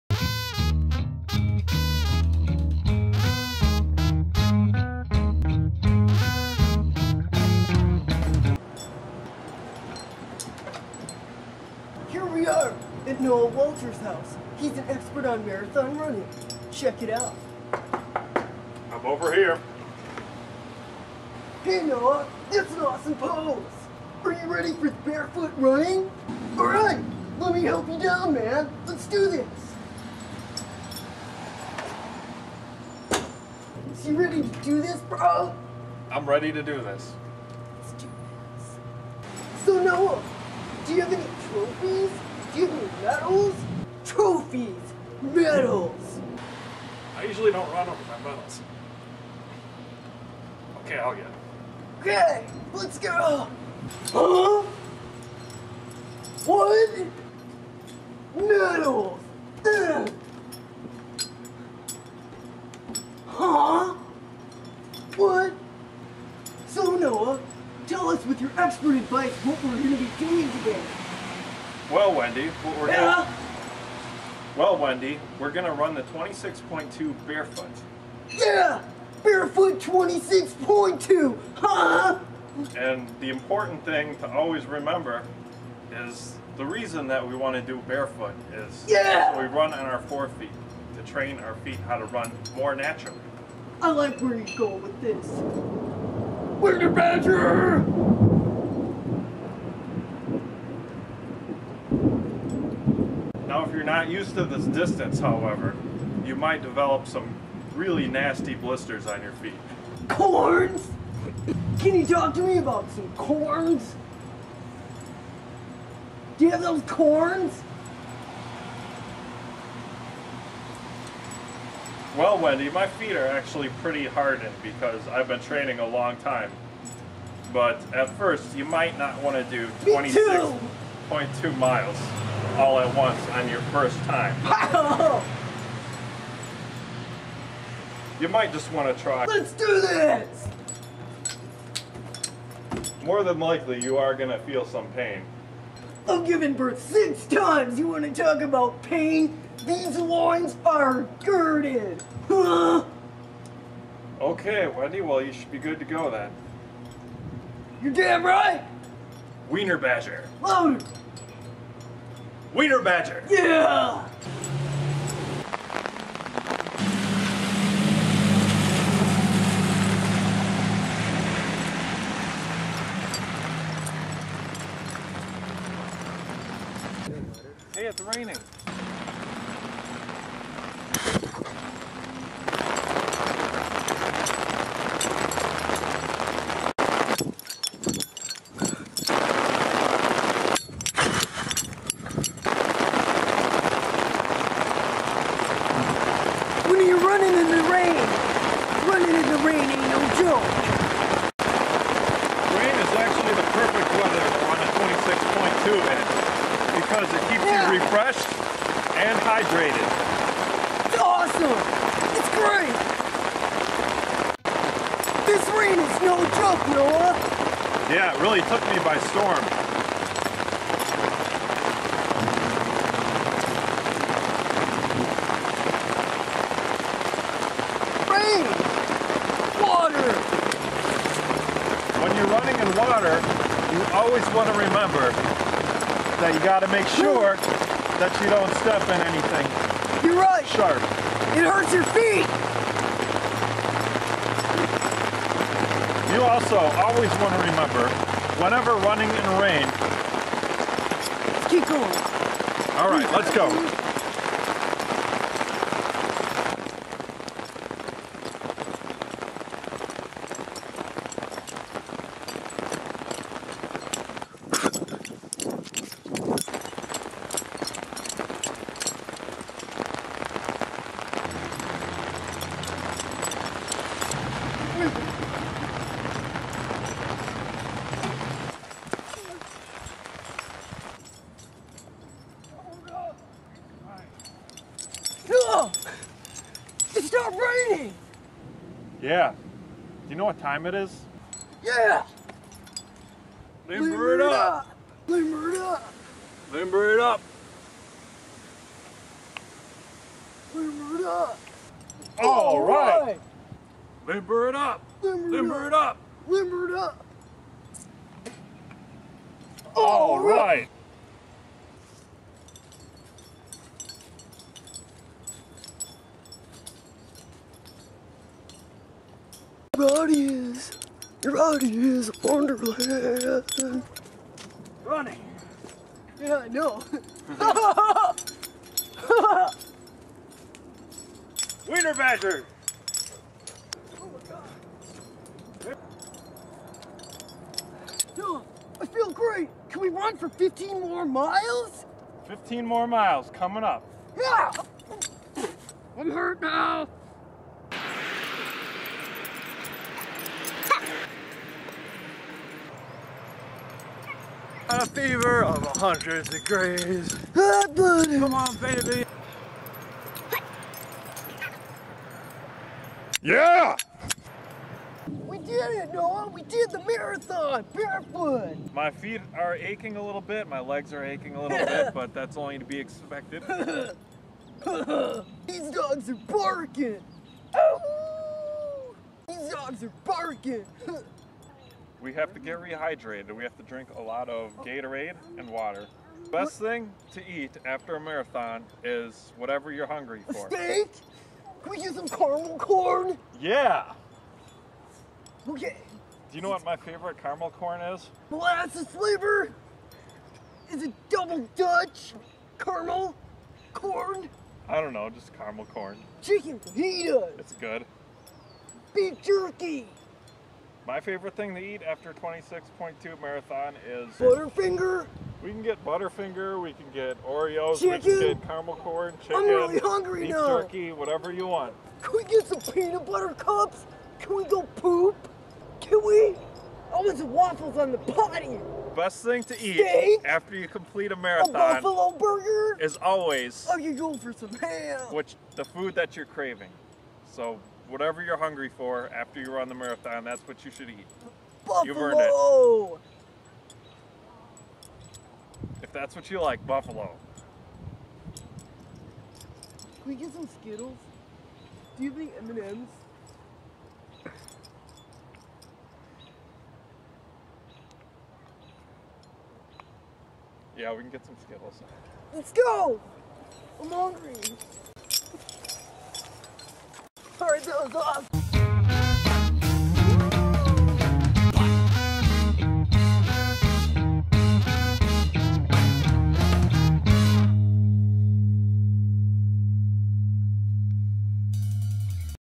Lesson Three "Barefoot Running" Wendy Weenerbadger's Fitness Kick
Wendy Weenerbadger and Noah Walter teach you how to run a marathon Barefoot